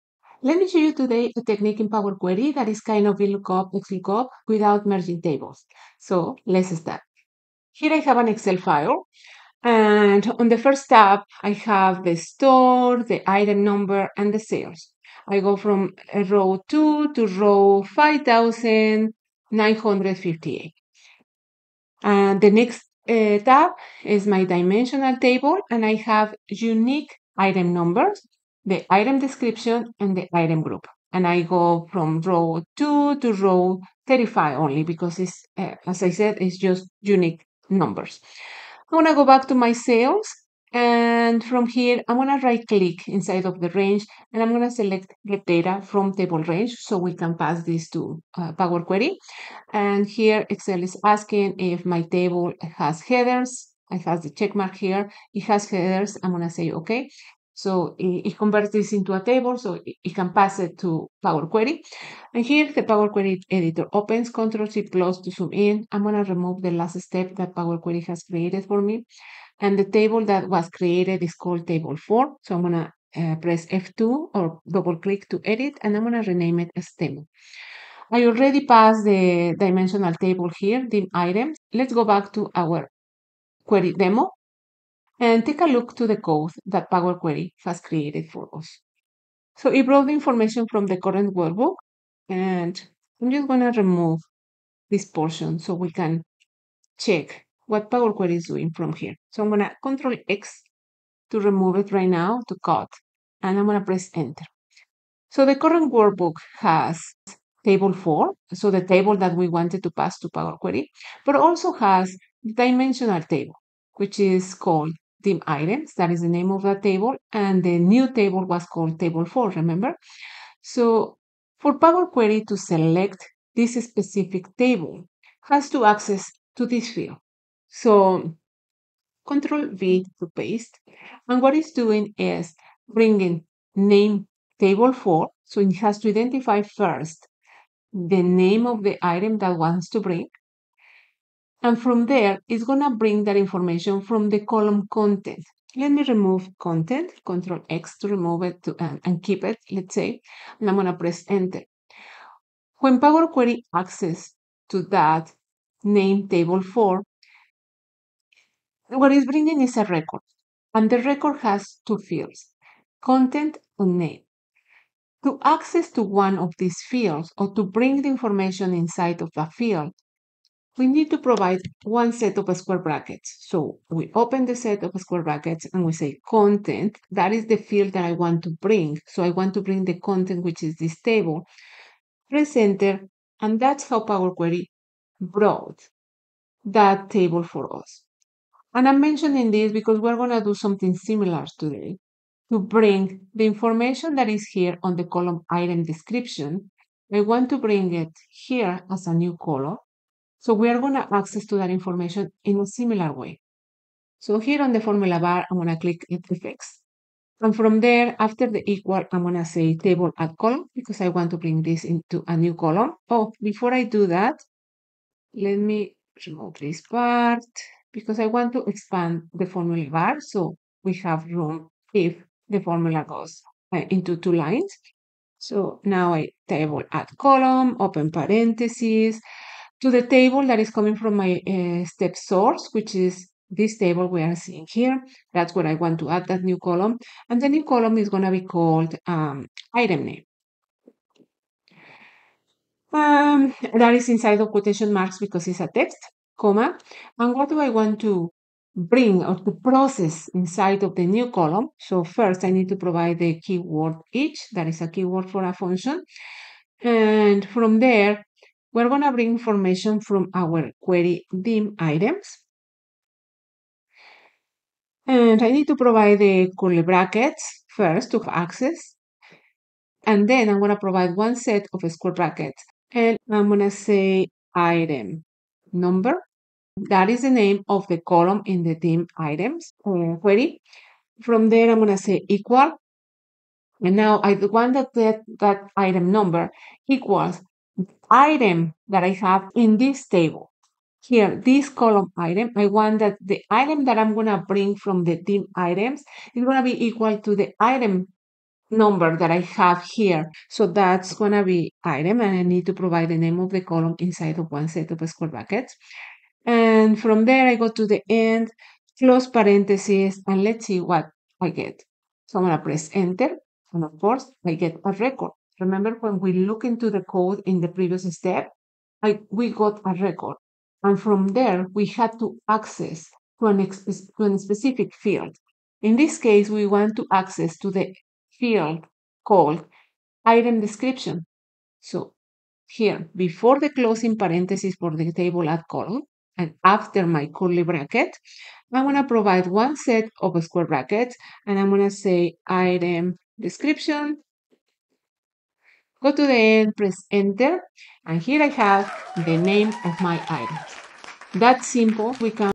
For Small Data Sets - VLOOKUP/XLOOKUP in Power Query Without Merging Tables
Please note that I have received feedback on the performance with this technique, as slow.
I have used it with small data sets like in the example that I presented, and works fine.
But, please keep that in mind.